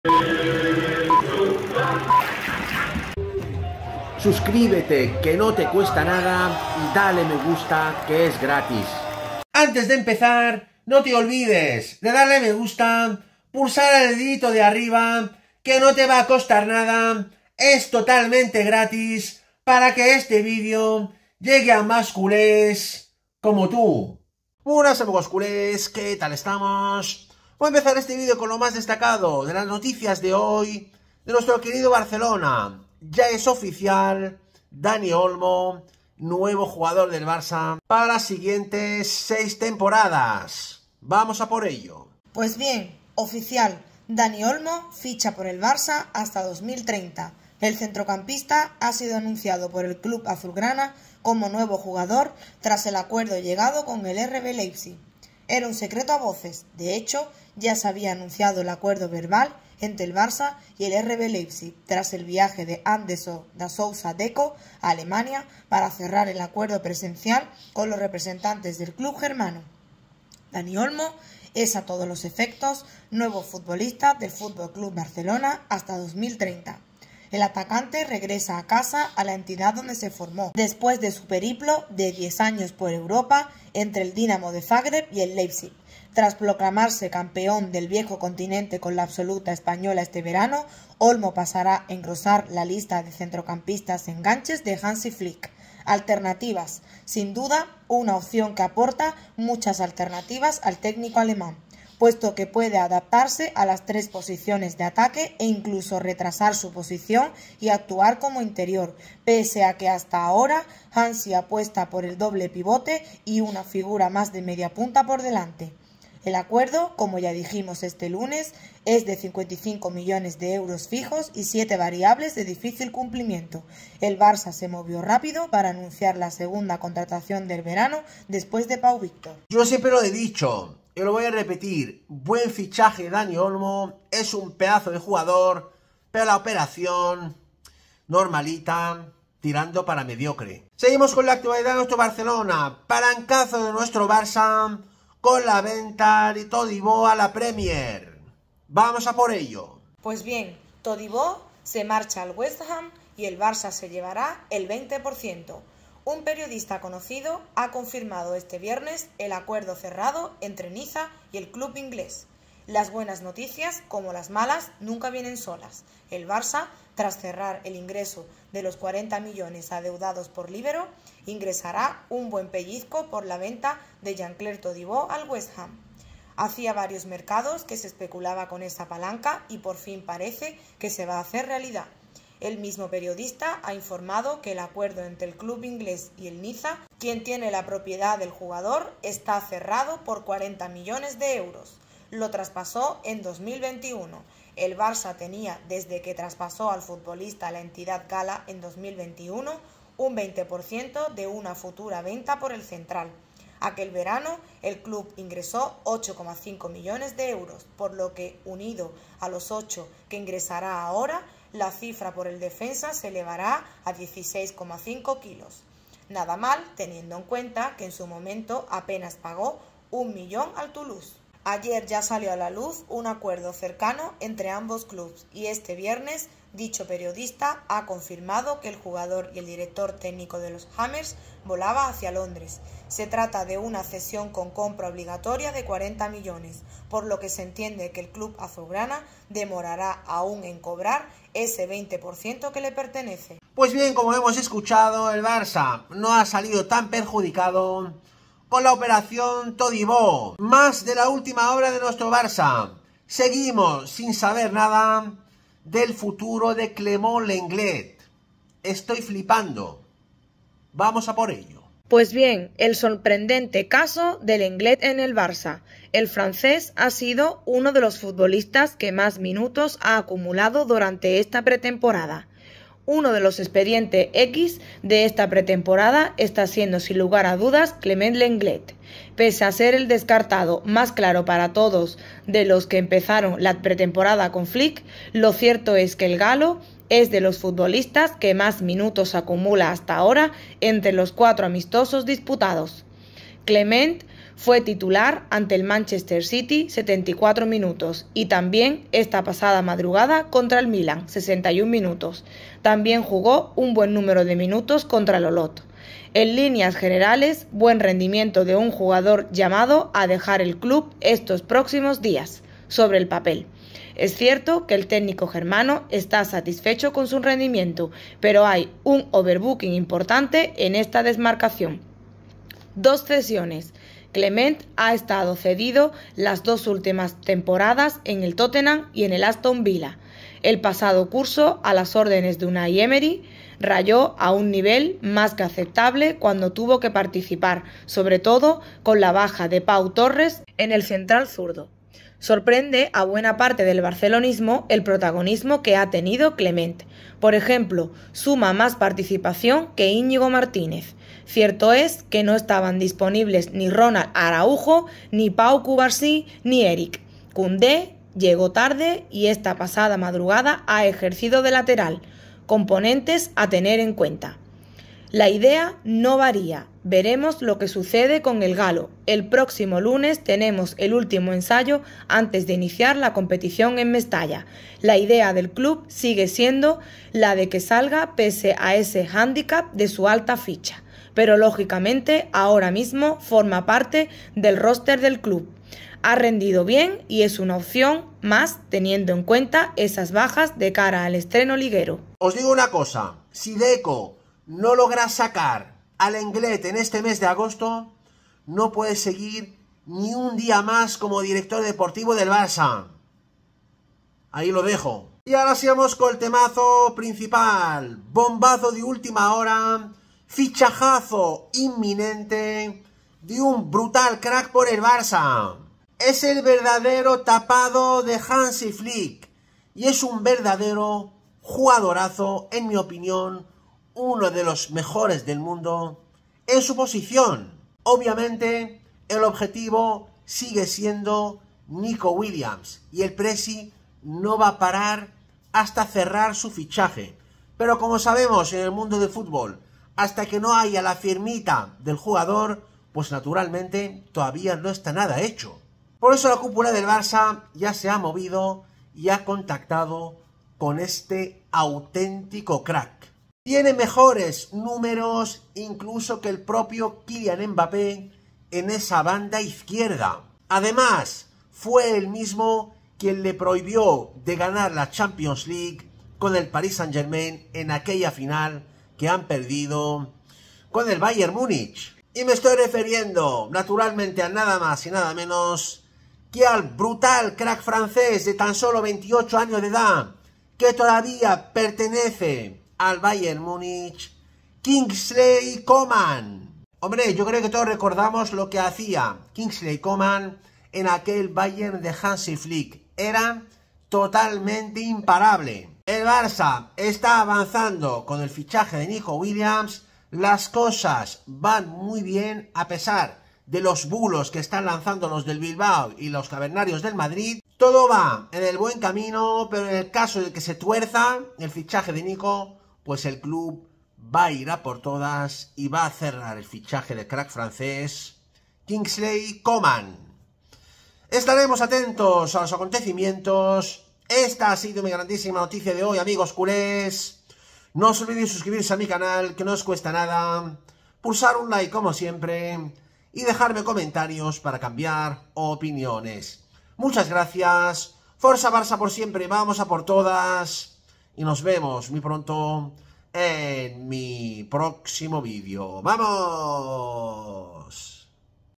¡MENUDO BOMBAZO! ¡FICHAJAZO INMINENTE DE ESTE BRUTAL CRACK POR EL BARÇA! ERA EL GRAN TAPADO DE FLICK
El Barcelona está a un paso de cerrar el fichaje de un brutal crack mundial para las siguientes cinco temporadas, y es el verdadero tapado de Hansi Flick